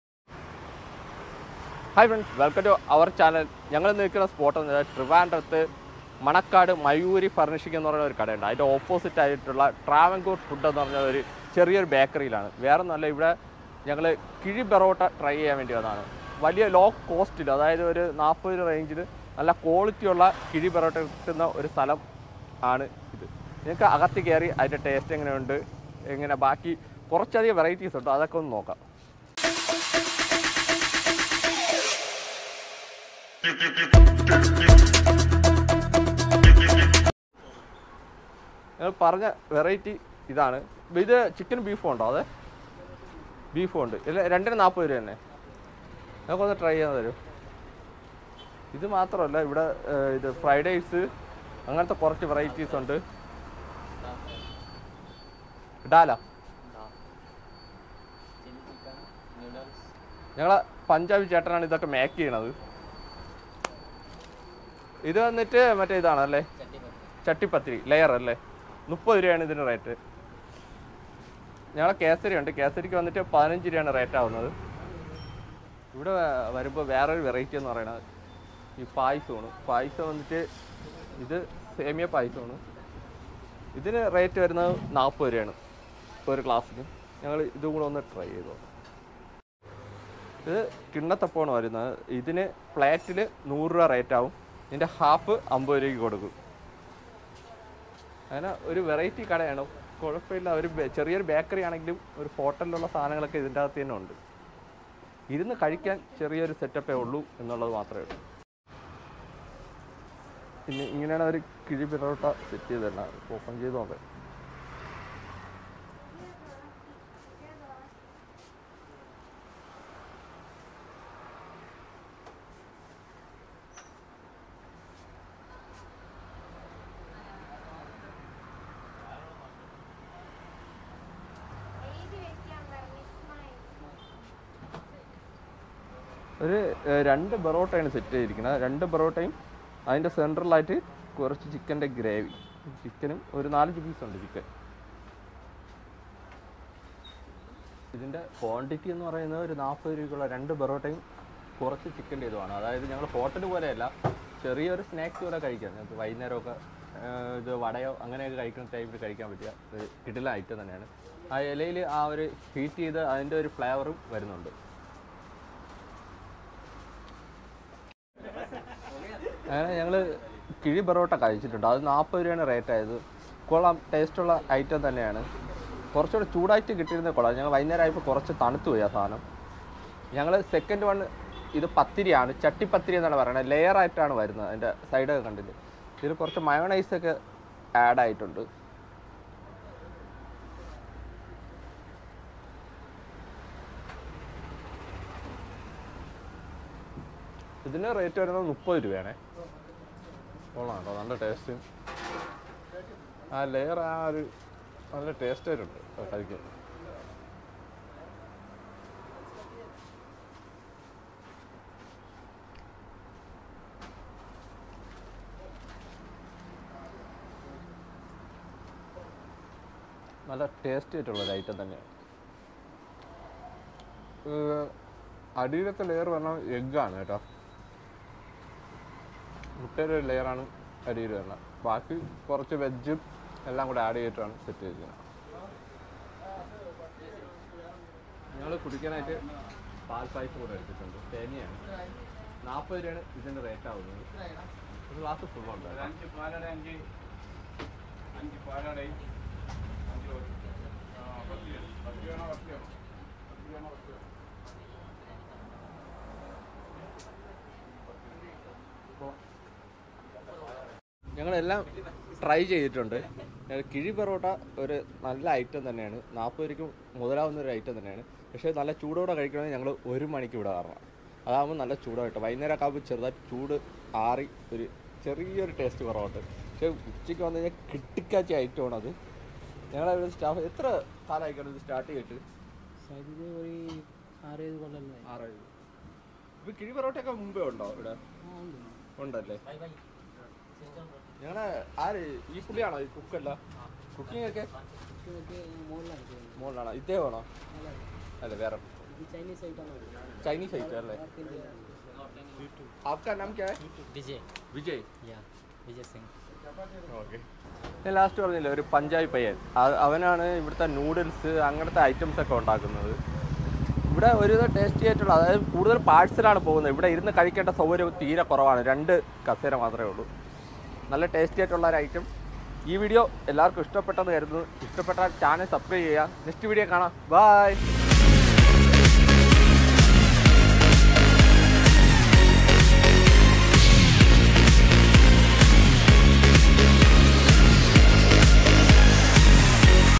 40 രൂപയ്ക്ക് കിഴി പൊറോട്ടയോ 😳 || മണക്കാട് 📍|| trivandrumfood || food || chicken
മണക്കാട് മയൂരിൽ ഫർണിഷിന് ഓപ്പോസിറ്റ് ഉള്ള കടയാണ് നല്ല ടേസ്റ്റ് ഉള്ള കിഴി കിട്ടുന്ന ഒരു കടയാണിത്